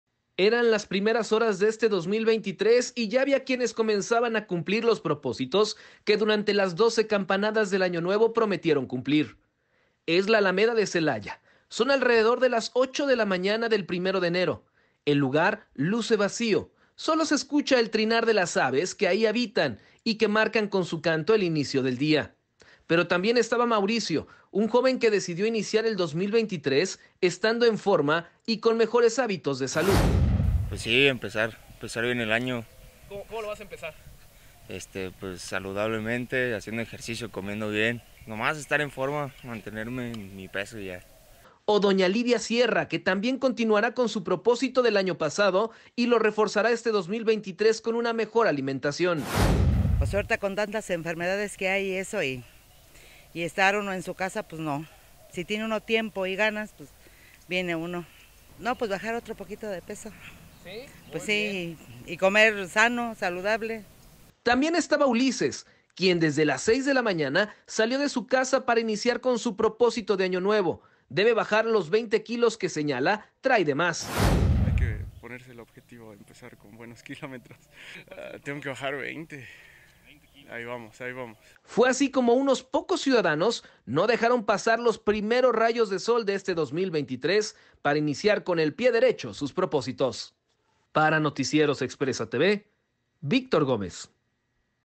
🔴🎥🏃 Inician el 2023 cumpliendo sus propósitos
Eran las primeras horas de este 2023 y ya había quienes comenzaban a cumplir los propósitos que durante las 12 campanadas del Año Nuevo prometieron cumplir…

Es la alameda de Celaya, son al rededor de las 8am del primero de enero, el lugar luce vacío, solo se escucha el trinar de las aves qué ahí habitan y que marcan con su canto el inicio del día…

Pero también estaba Mauricio un joven que decidió iniciar el 2023 estando en forma y con mejores hábitos de salud